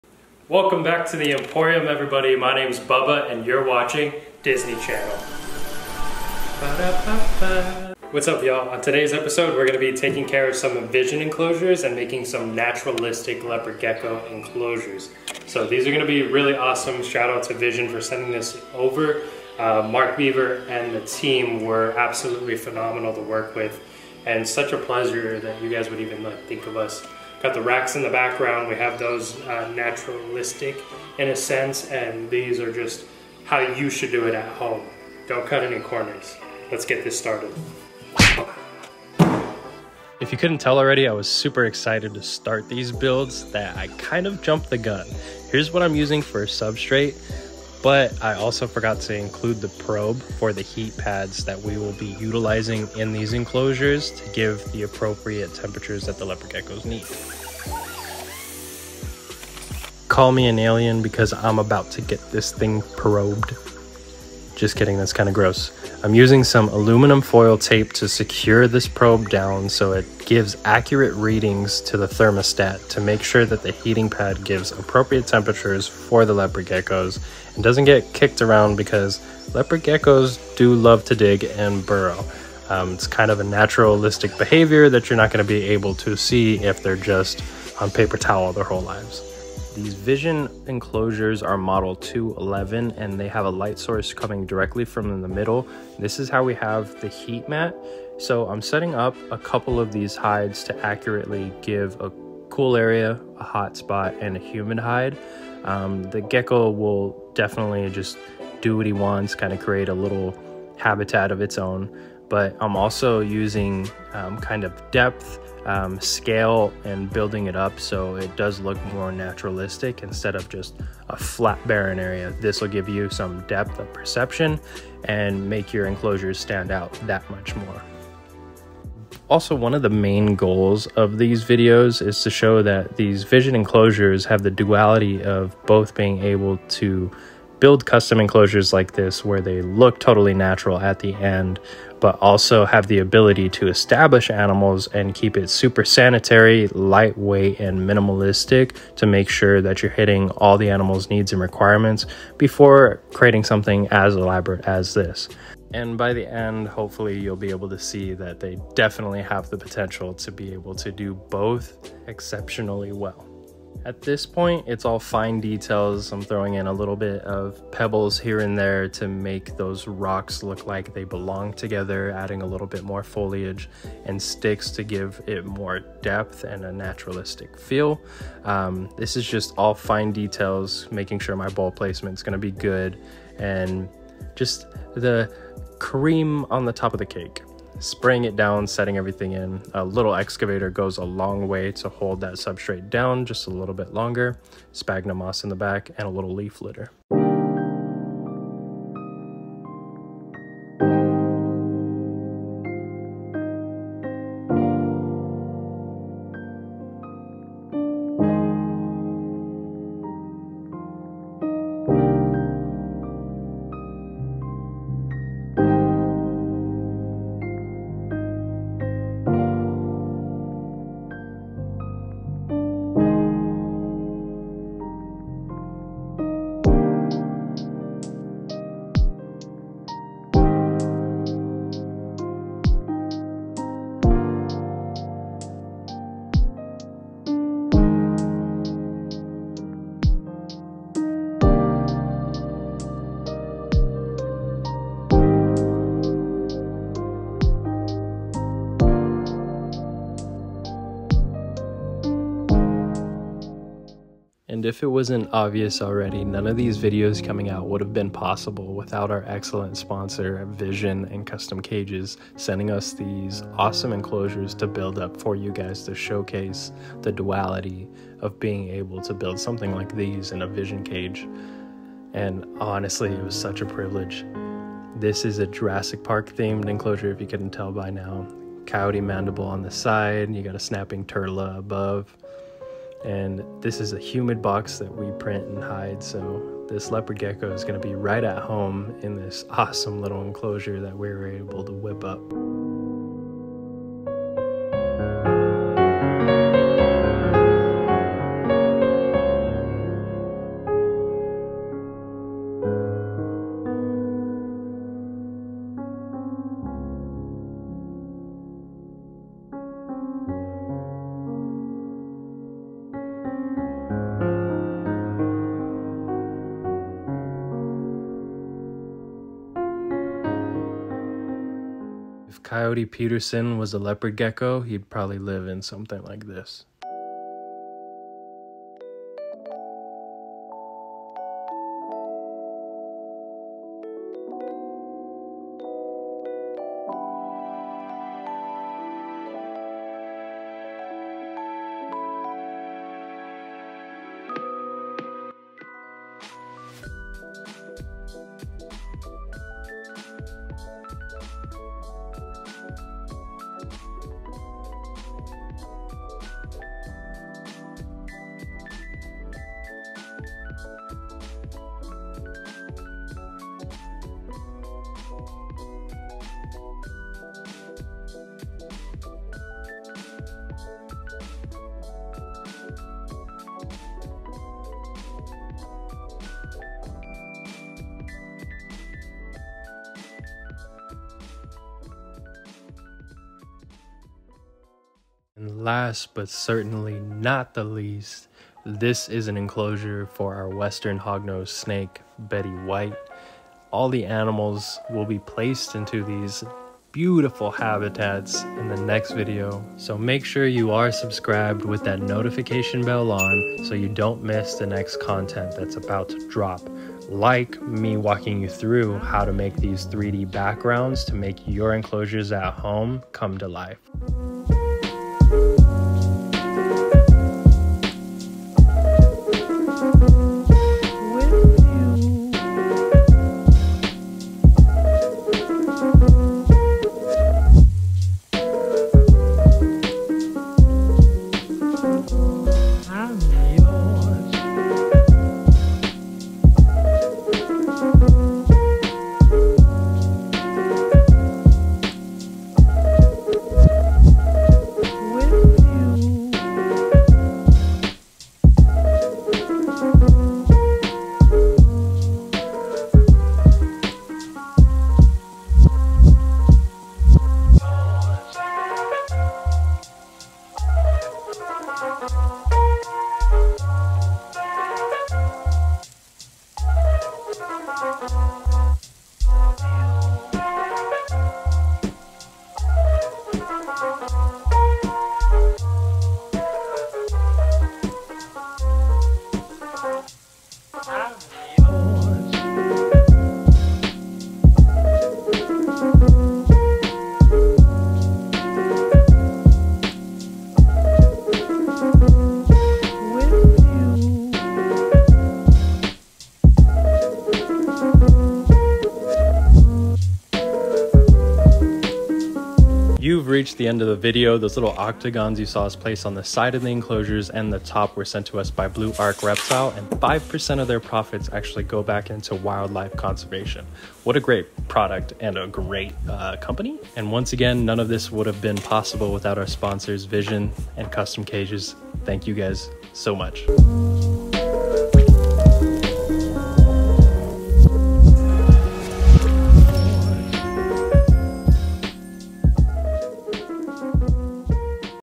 Naturalistic Leopard Gecko Enclosures of the Future!
This video was so much fun to film and put together for y’all!

Thank you to Jeff @JMGreptile1 for the amazing Hognose pictures and content (hog in power armor)
Follow Jeff on Instagram!

Shout out to my fallout fiends who clicked merely for the thumbnail (like Grayson did)

Thank you guys for stopping by!

Follow us and keep up to date on our social medias